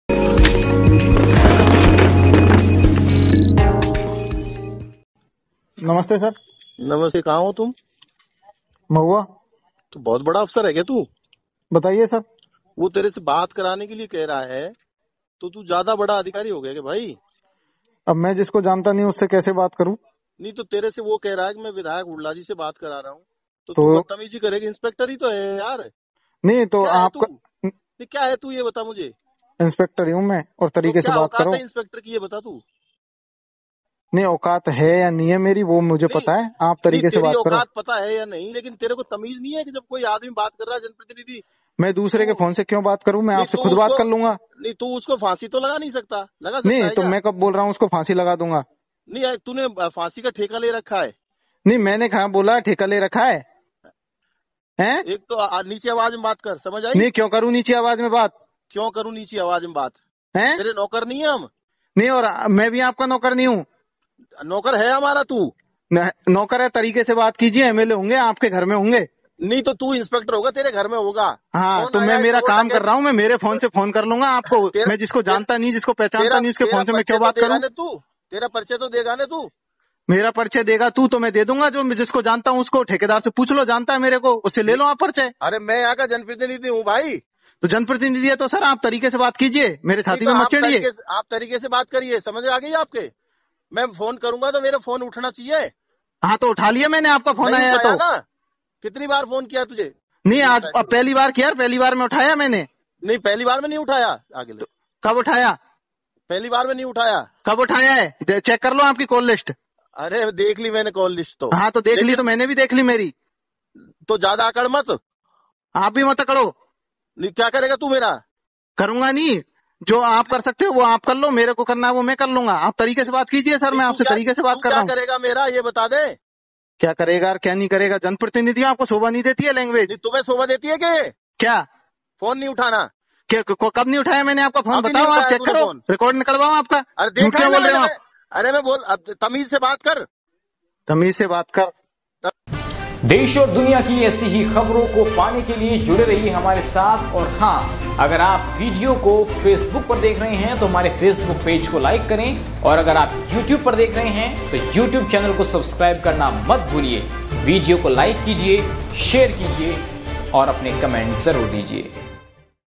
MLA ने Phone पर Inspector को धमकाया तो Inspector ने निकाली MLA की हवा हो गई बहस !
MLA ने Phone पर Inspector को धमकाया तो Inspector ने निकाली MLA की हवा हो गई बहस ! Watch Out Full Video And News !